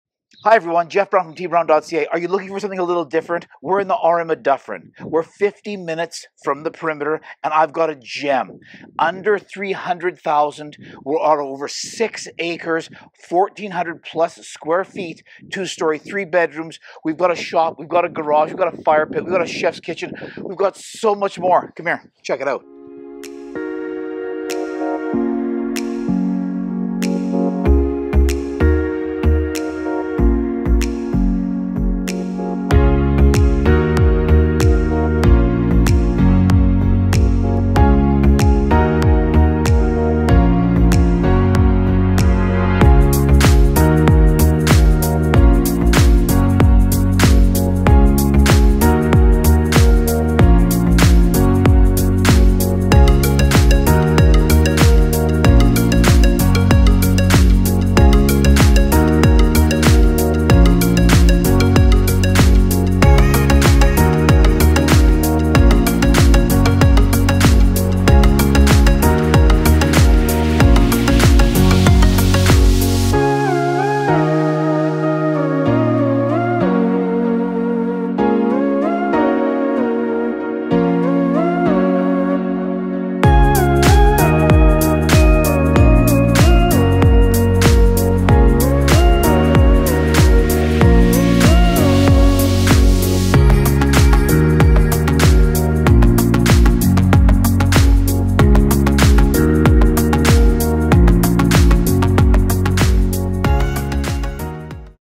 FOR SALE - 39083 38 Road W, Dufferin, MB - $299,900
Showings start on Thurs, Mar.24th. If you want to be surrounded by peaceful views all day and night long then this stunning 3 bedroom two storey on 6.03 acres is the one for you! Large chef's kitchen opens to the dining area and patio doors to the beautiful never ending treed lot with fire pit area and garden. Fantastic living room surrounded by windows and the entrance to the front open porch with southern exposure where you will see the most amazing views over the wonderful farmer's field. The upper level features a huge 4 pc bathroom with heated flooring, all 3 bedrooms including the great sized primary room with walk in closet. Oversized mudroom and laundry area, solid core doors, central a/c, central vac, HRV, plenty of storage, fantastic Xplornet internet access, high end security with the Vivint alarm and Hardie Board exterior. Double detached 20 x 30 garage with 19.5 x 11 heated shop inside. All of this located only a few minutes from amenities and only 50 mins from the perimeter. You will love it!

Sutton Group - Kilkenny Real Estate